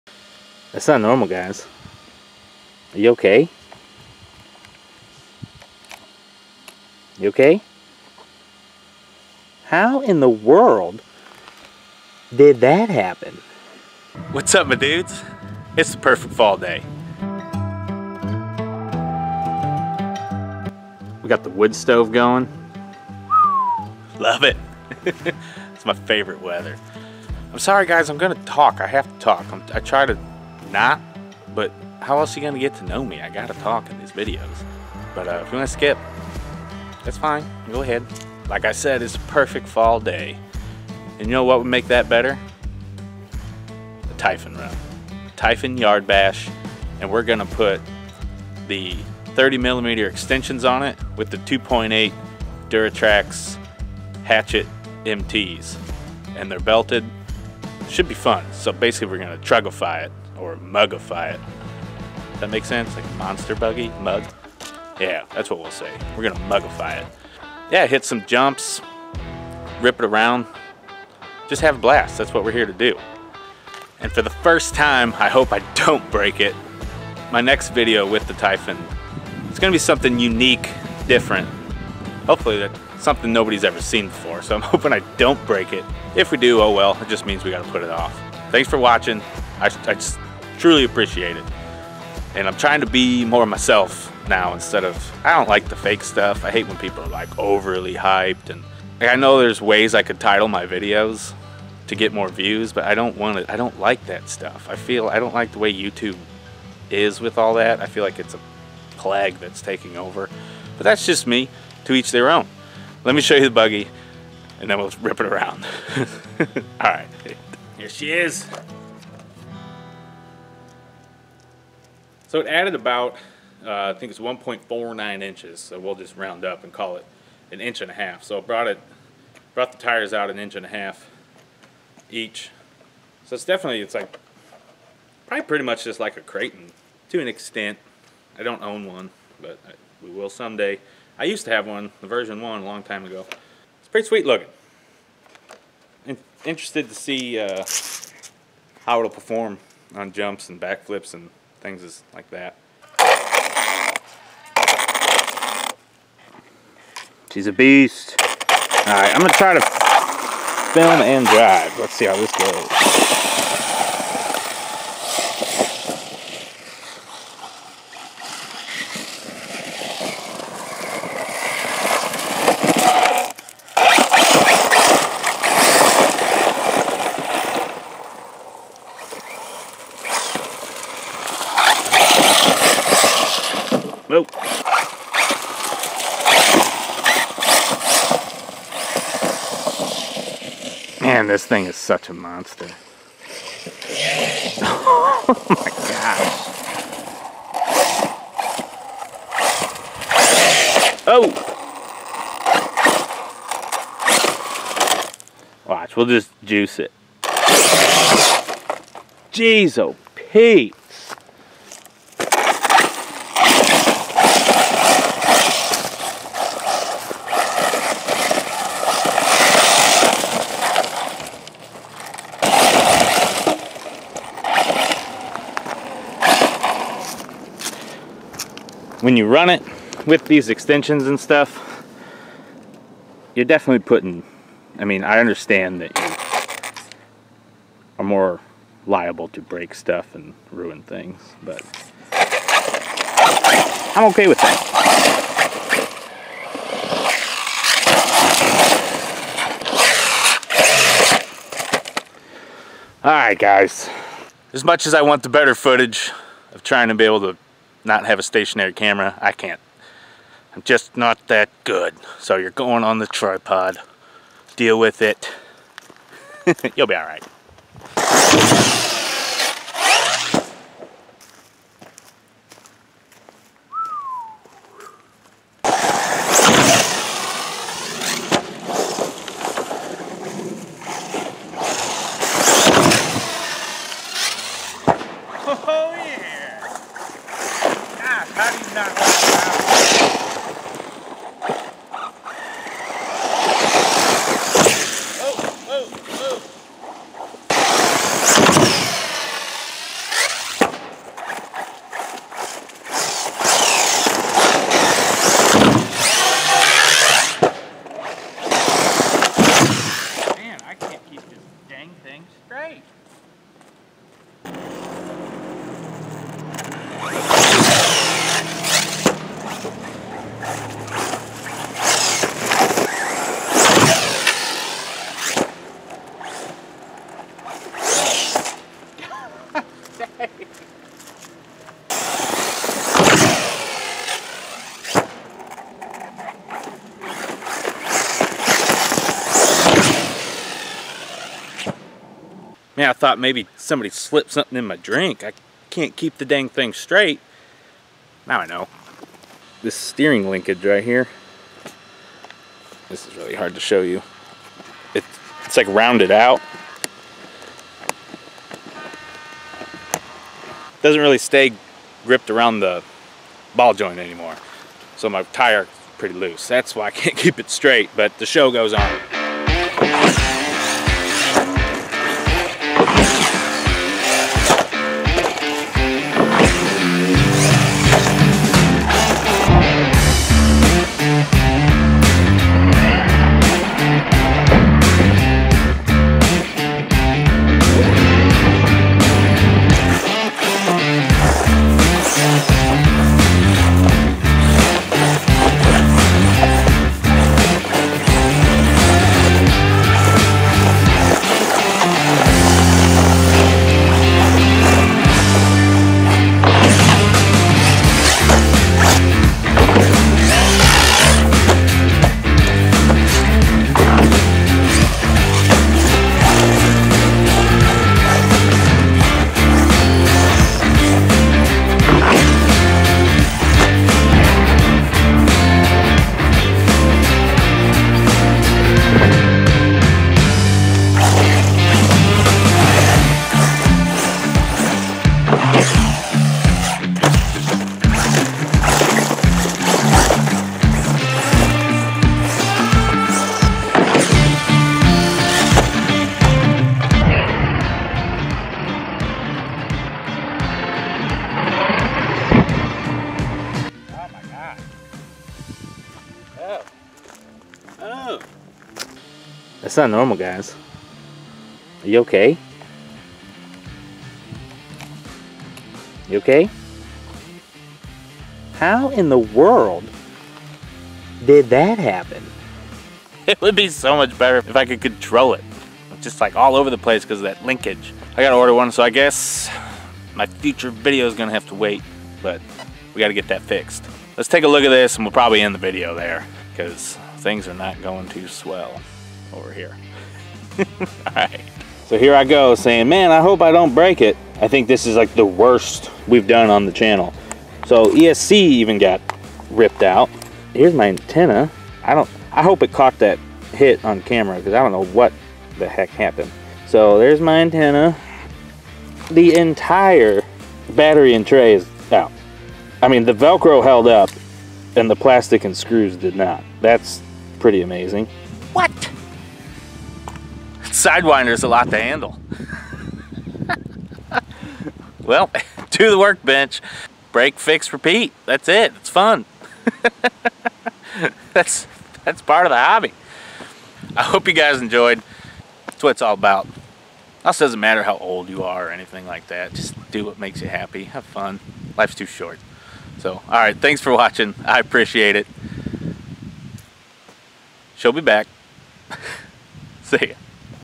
Typhon Castle Hatchet! I Broke It This time..(Arrma Typhon 6s BLX)
What's up y'all? In this video we take the Arrma Typhon 6s BLX V5 and install some 30mm Hub extensions with Duratrax Hatchet 2.8 MTs. For the first time I try not to break it, and well that didn't go well. Oh well, its all part of the fun. I hope you enjoyed and I'll see ya next time.